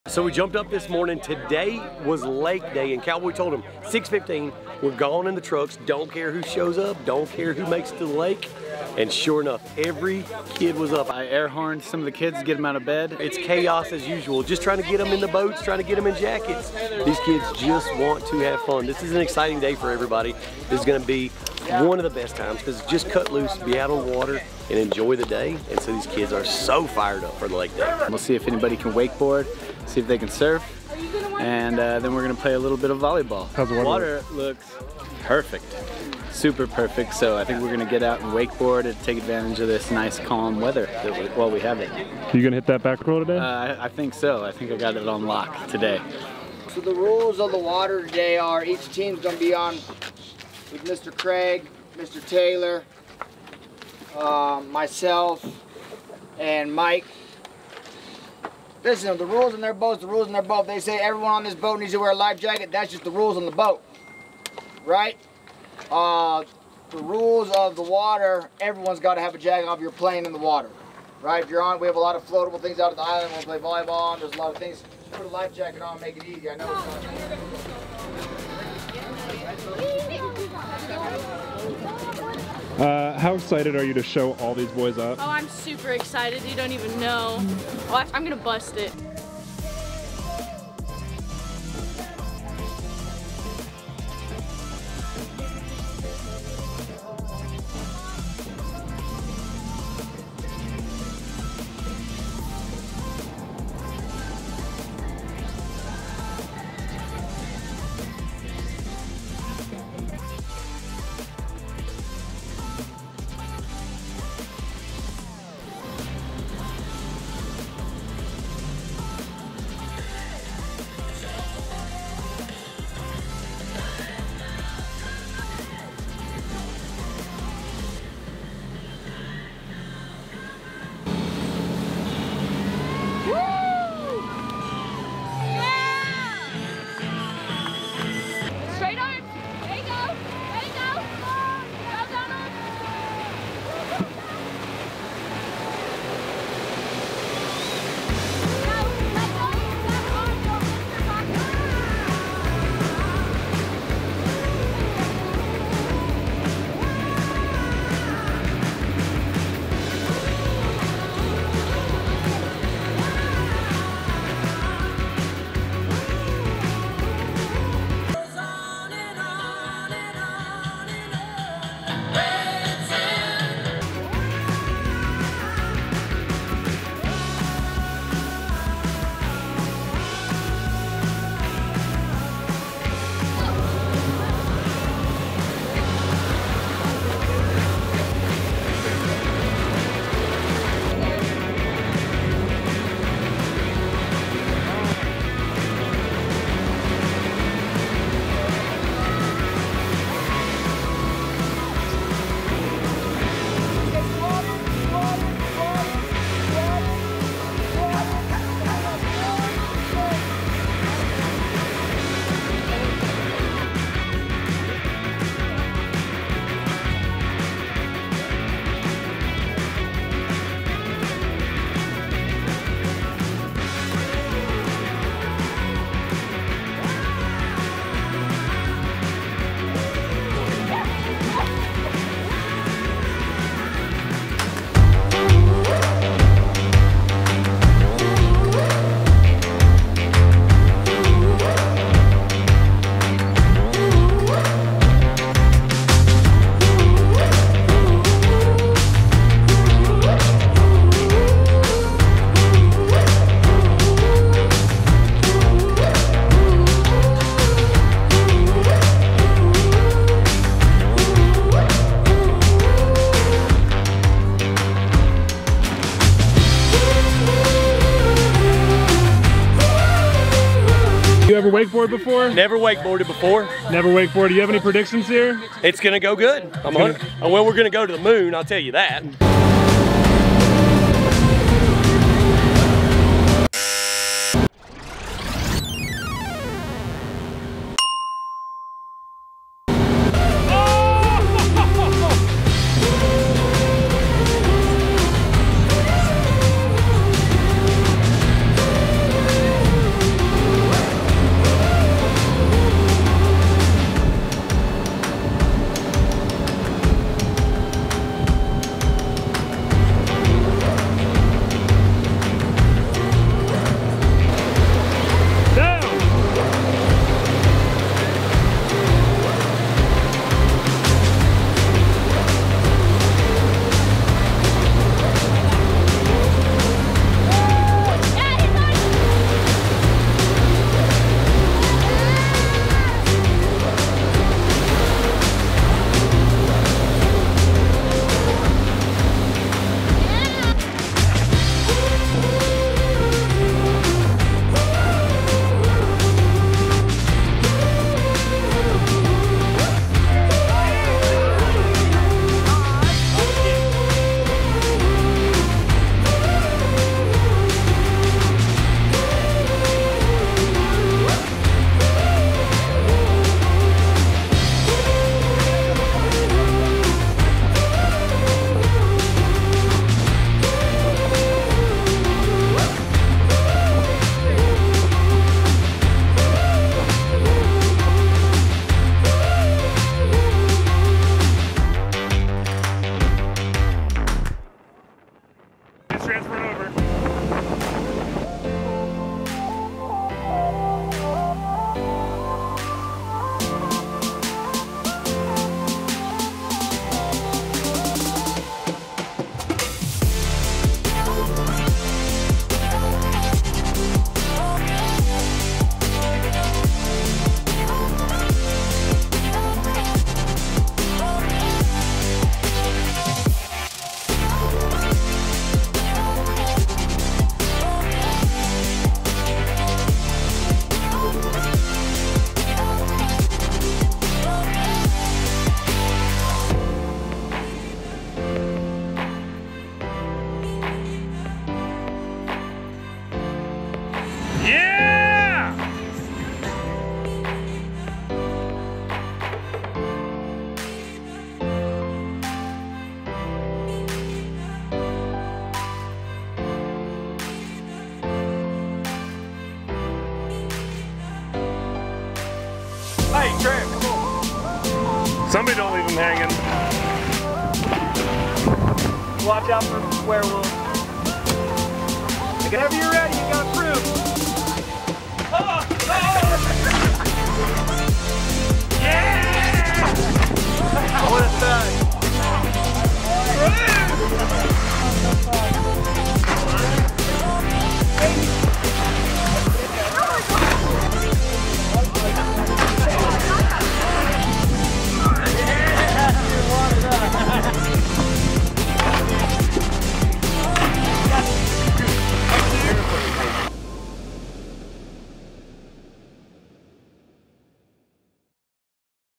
Teachin’ the kiddos the meaning of Ride or Die - Lake Day
It’s lake day! Big thanks to @HyperliteWakeboards on this one for getting the kids jacketed up and keeping em safe.